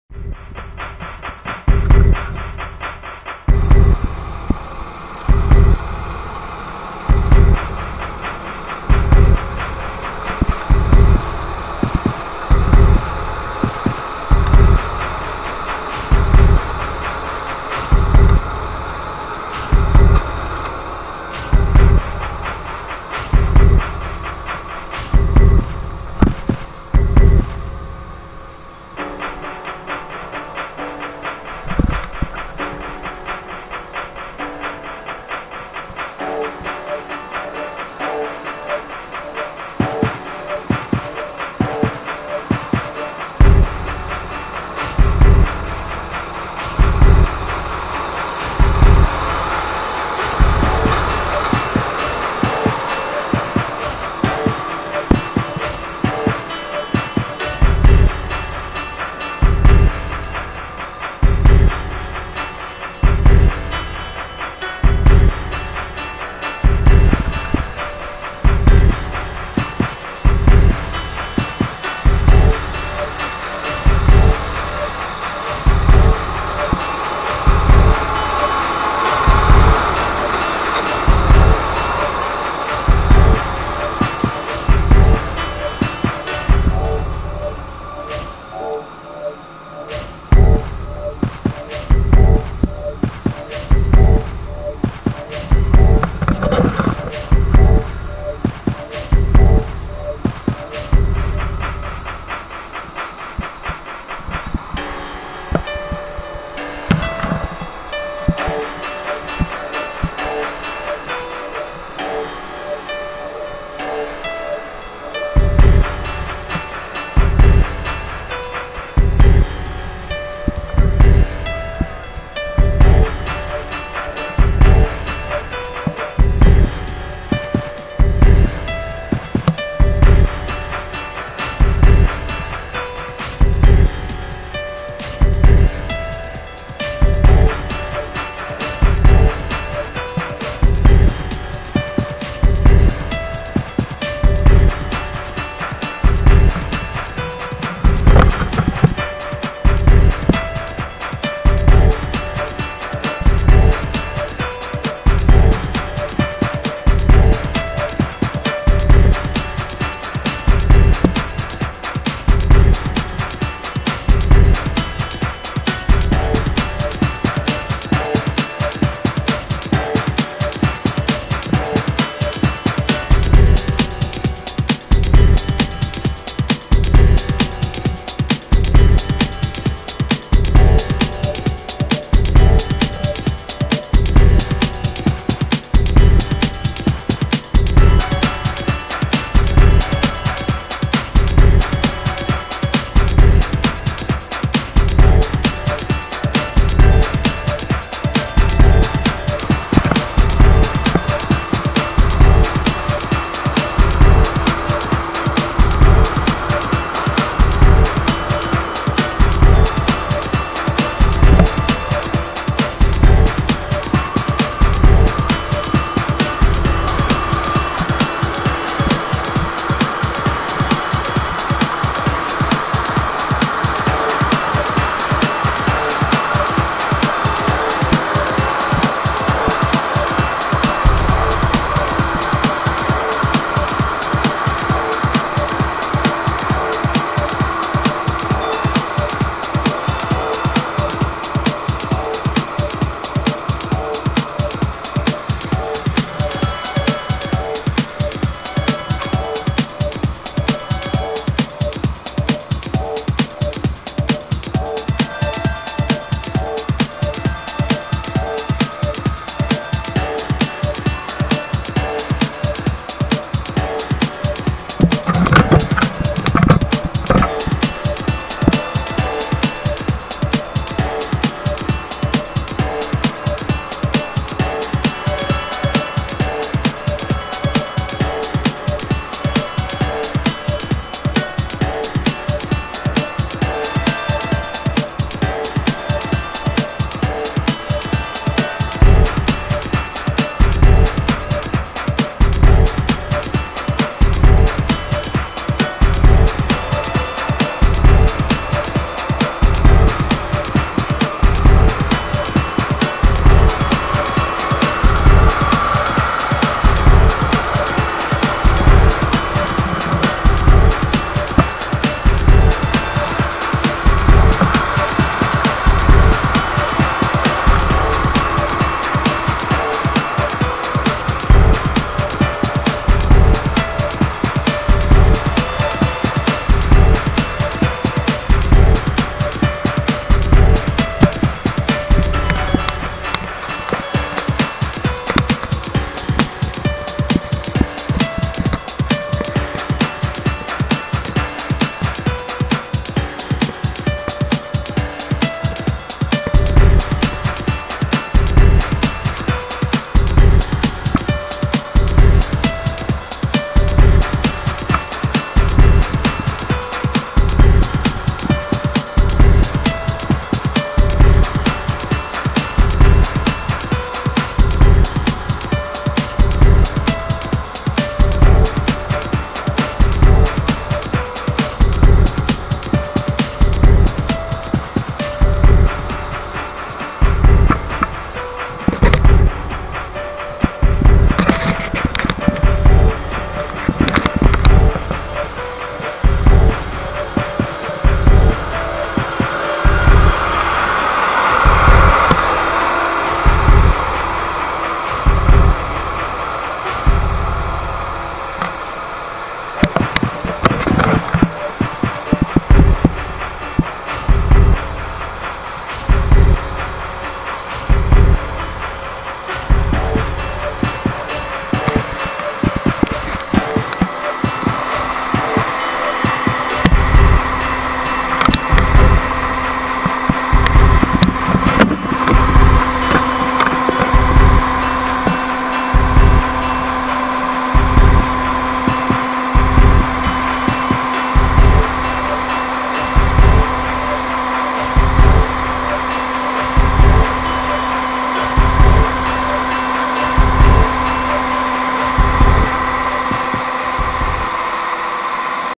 YANMAR YH6115 超湿田突破 トロトロ圃場稲刈り モミロマン飼料米
モミロマン飼料米刈取
ウンカ
分草パイプ3本